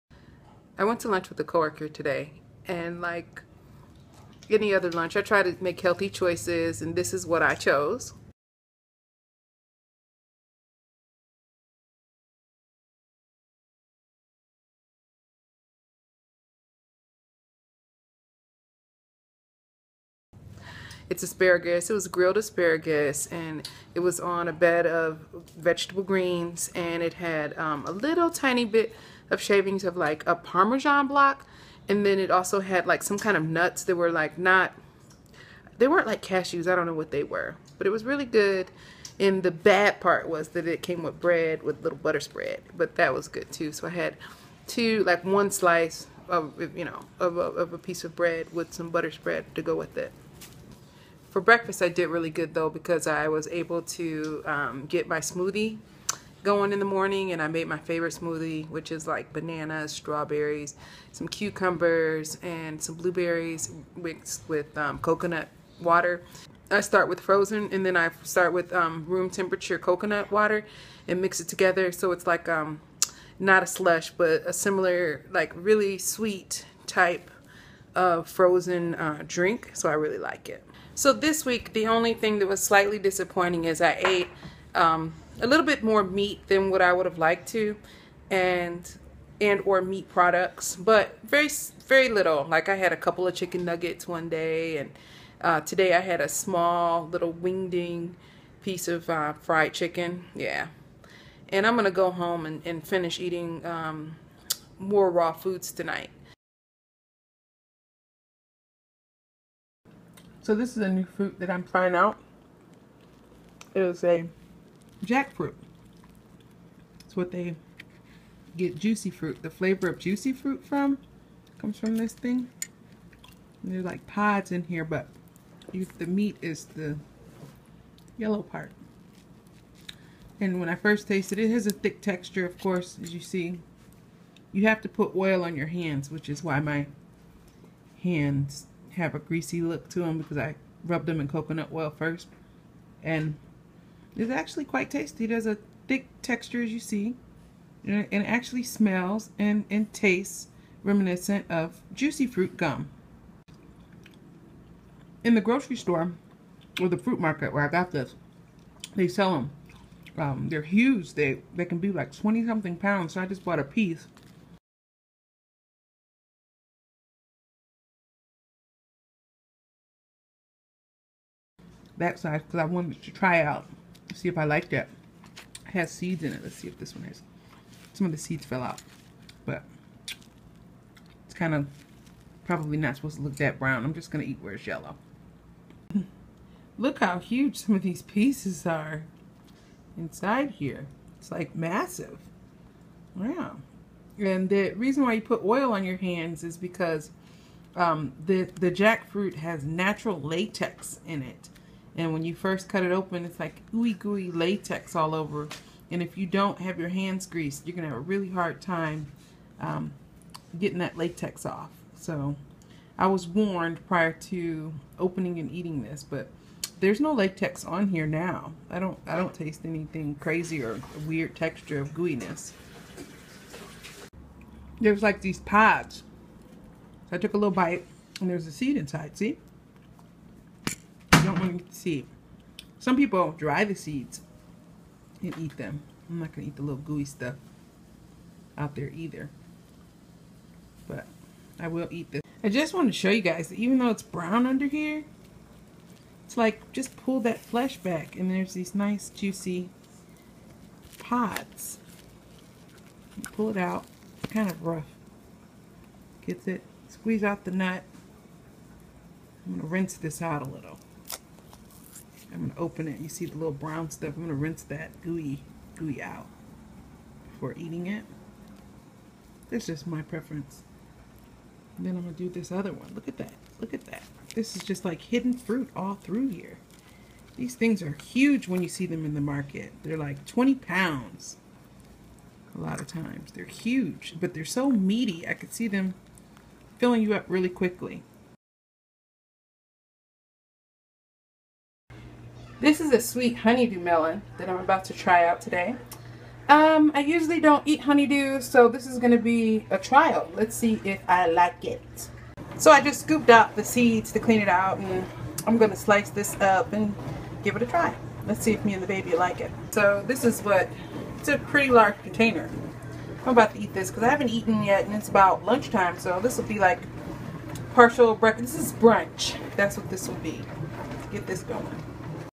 Eating Jackfruit for the 1st time..Eating out with co-workers and weight loss update
Trying a Jackfruit for the first time. How to eat a jackfruit... Healthy choices while eating with co-workers and friends. Trying a honeydew melon and more!